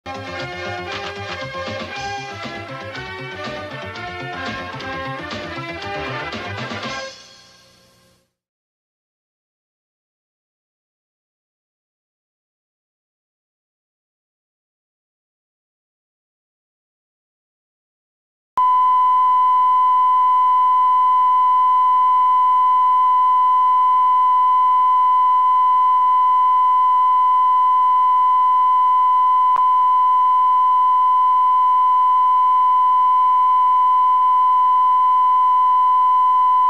Заставка и часы - Аминаку . 1994
Записано во время конца эфира. Не баньте пж.